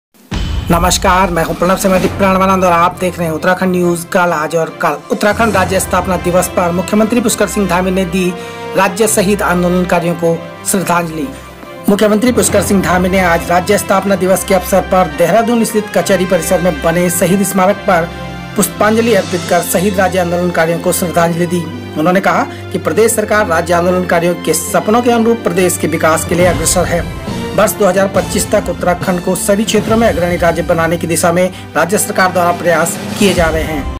मुख्यमंत्री पुष्कर सिंह धामी ने शहीद राज्यआंदोलनकारियों को श्रद्धांजलि l Uttarakhand News l
Dosto es video mai ham Uttarakhand rajya sthapna divas par mukhya mantri Pushkar Singh Dhami dwara rajya andolankariyo ko shradhanjali dene ki news janege.